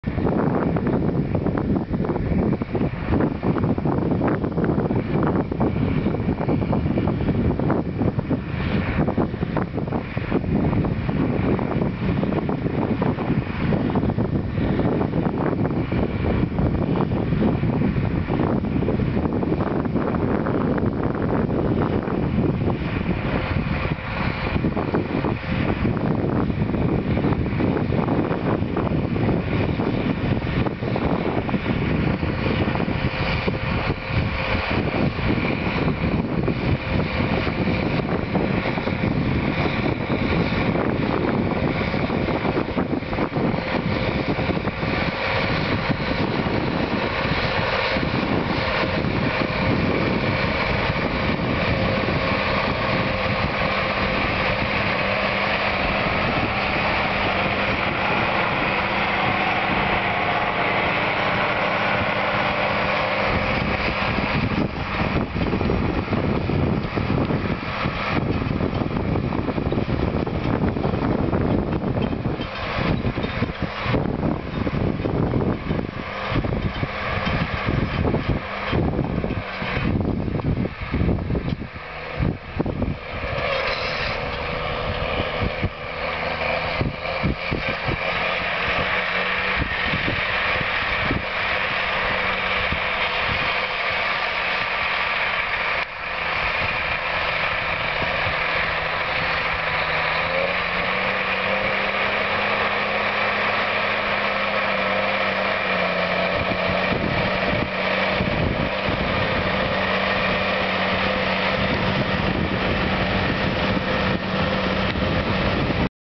Case IH Magnum 245 and Kuhn Manager Plough
50km/h Magnum with front suspension, AFS and Accuguide Auto Steer ploughing wheat ground at Valetta Farms In Mid Canterbury, New Zealand with a 7 furrow Reversable Kuhn Wagon Plough.